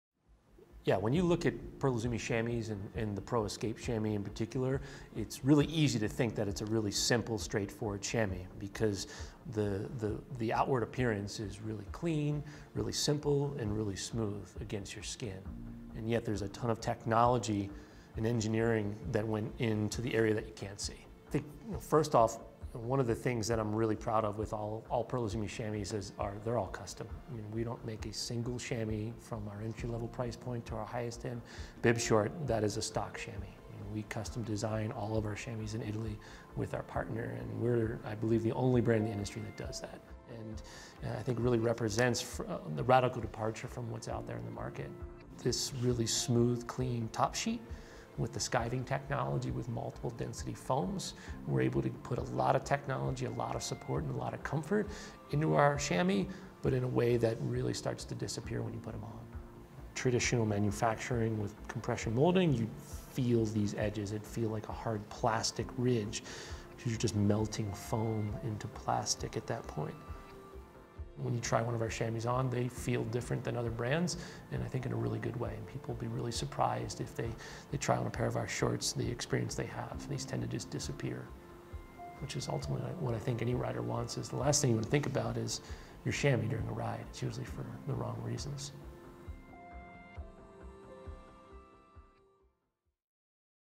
PEARL iZUMi Craft Series – 1:1 Chamois
We work hard to make sure your chamois is the last thing you'll think about on your bike. Learn more about the core principals we use to make the best chamois possible.

Our 1:1™ Chamois with floating top sheet design delivers unmatched comfort.